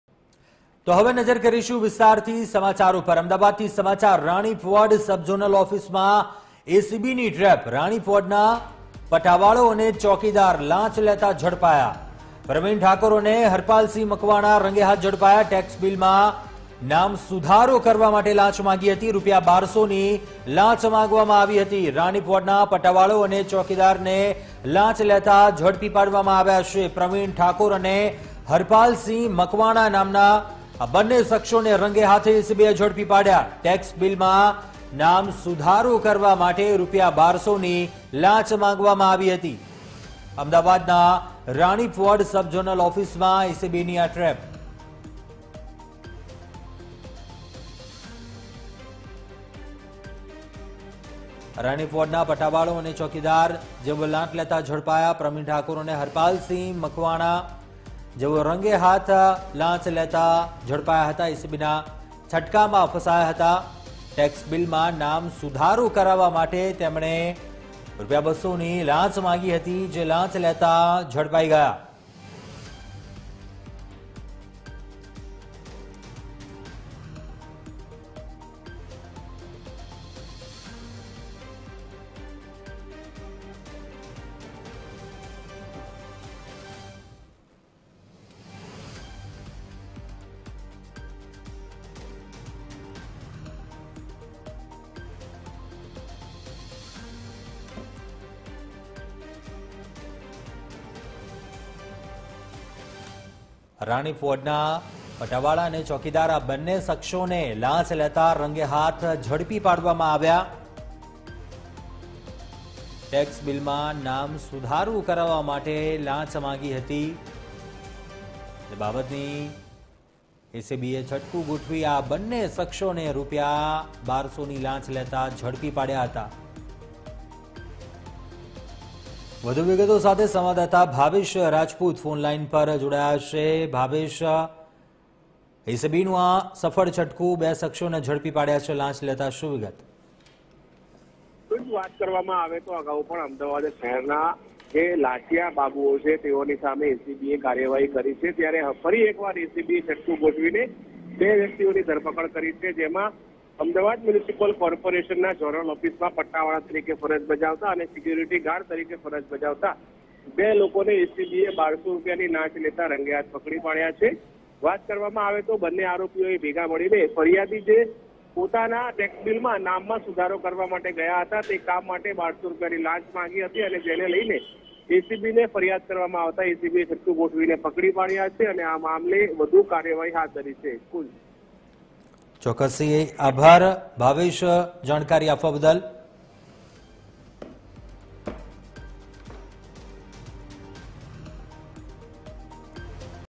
Ahmedabad : રાણીપ વોર્ડ સબ ઝોનલ ઓફિસમાં એસીબીની ટ્રેપ
રાણીપ વોર્ડના પટ્ટાવાળો અને ચોકીદાર લાંચ લેતાં ઝડપાયા
પ્રવિણ ઠાકોર અને હરપાલસિંહ મકવાણા  રંગેહાથ ઝડપાયા
ટેક્સ બીલમાં નામ સુધારો કરાવવા 1200ની માંગી હતી લાંચ